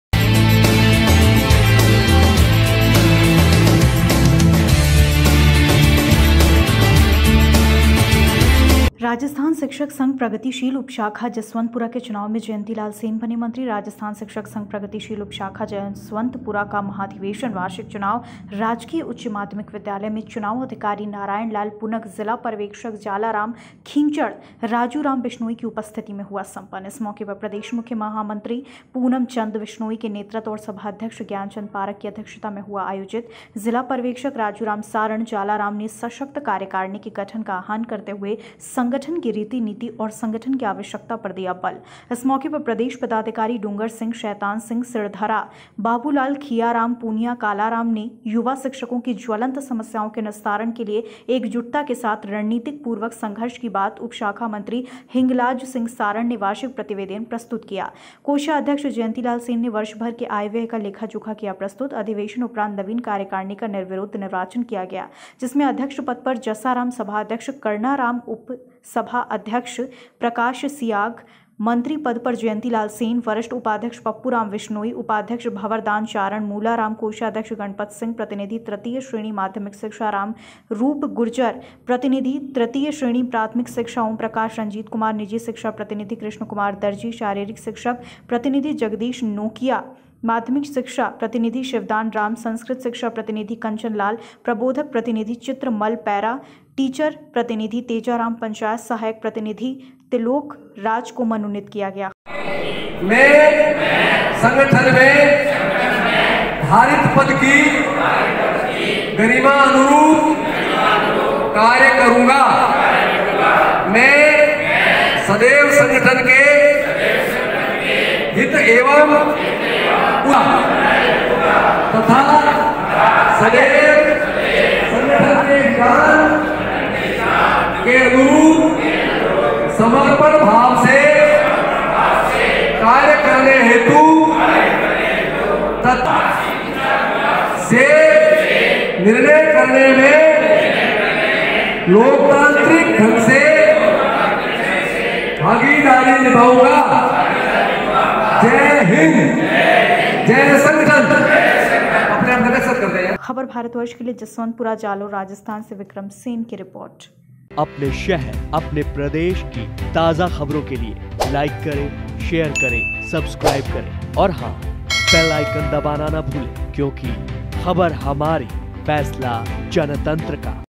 जसवंतपुरा(जालोर):राजस्थान शिक्षक संघ प्रगतिशील उपशाखा जसवंतपुरा के चुनाव में जयंतीलाल सेन बने मंत्री
अधिवेशन उपरांत नवीन कार्यकारिणी का निर्विरोध निर्वाचन किया गया
प्रदेश मुख्य महामंत्री पूनमचंद विश्नोई के नेतृत्व व सभाध्यक्ष ज्ञानचंद पारख की अध्यक्षता में आयोजित
          रणनीति पूर्वक संघर्ष की बात उपशाखा मंत्री हिंगलाज सिंह सारण ने वार्षिक प्रतिवेदन प्रस्तुत